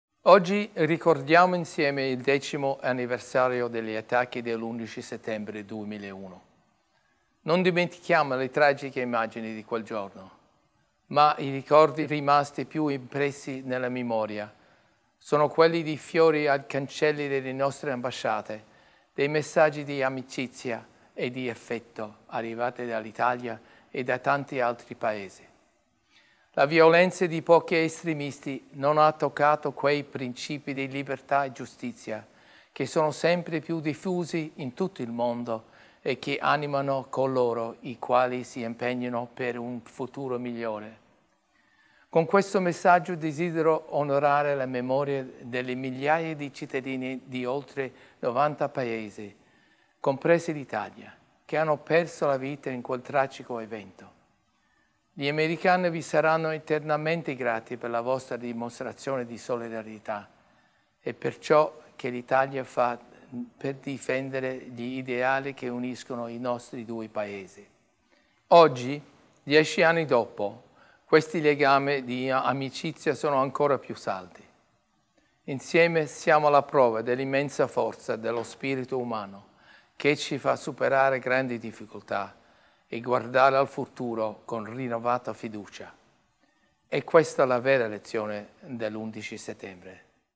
L'Ambasciatore David Thorne commemora l'11 settembre
"I ricordi rimasti più impressi nella memoria sono quelli dei fiori ai cancelli delle nostre ambasciate, dei messaggi di amicizia e affetto arrivati dall'Italia e da tanti altri Paesi. Gli americani vi saranno eternamente grati per la vostra dimostrazione di solidarietà e per ciò che l'Italia fa per difendere gli ideali che uniscono i nostri due Paesi. Oggi, dieci anni dopo, questi legami di amicizia sono ancora più saldi."

Con queste parole l'Ambasciatore degli Stati Uniti David Thorne commemora il decimo anniversario degli attacchi dell'11 settembre 2001 e ringrazia gli italiani per la solidarietà dimostrata e per l'impegno nel difendere i valori condivisi dai due Paesi.

L'Ambasciatore rende omaggio "alla memoria delle migliaia di cittadini di oltre novanta Paesi, compresa l'Italia, che hanno perso la vita in quel tragico evento" e sottolinea che "la violenza di pochi estremisti non ha toccato quei principi di libertà e giustizia che sono sempre più diffusi in tutto il mondo e che animano coloro i quali si impegnano per un futuro migliore."